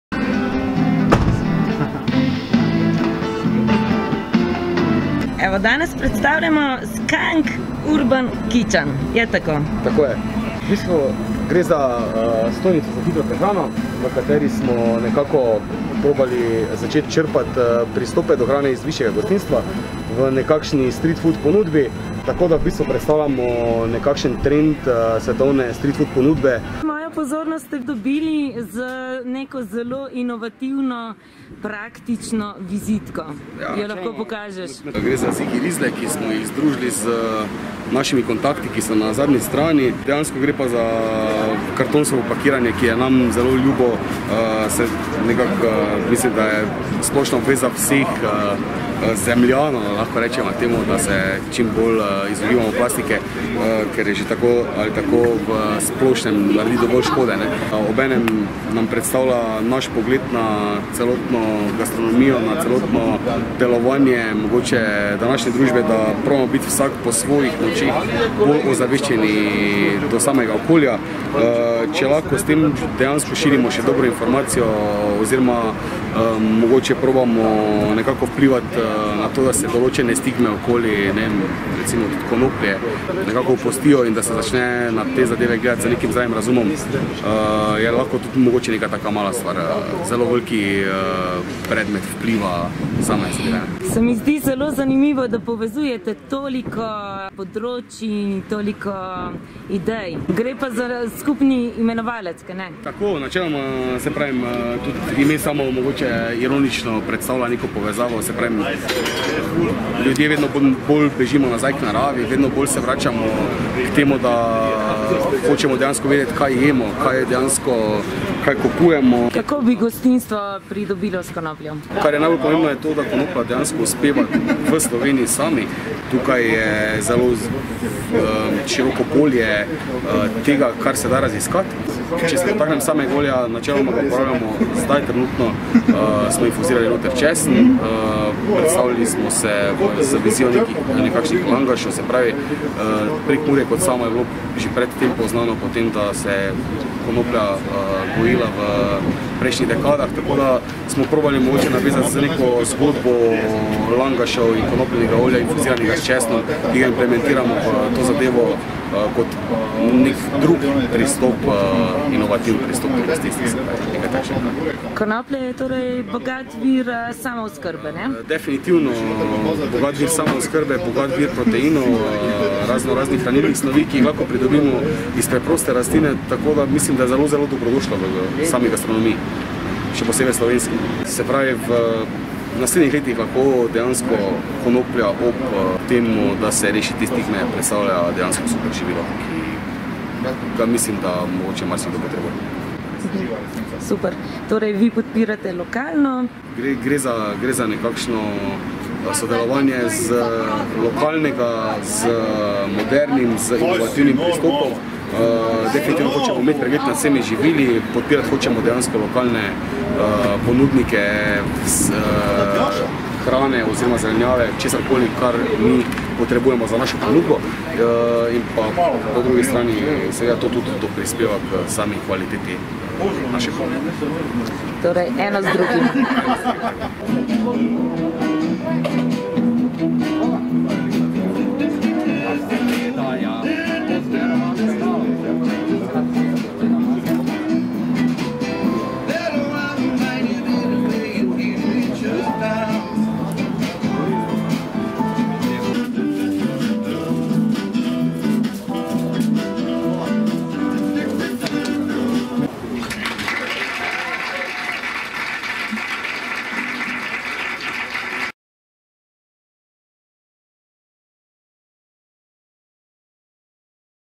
Kreativni s konopljo
S konopljo smo lahko kreativni in povezovalni na mnogih področjih.
Z ekipo Skunk Urban Kitchen iz 1. mednarodnega sejma športa in zdravja.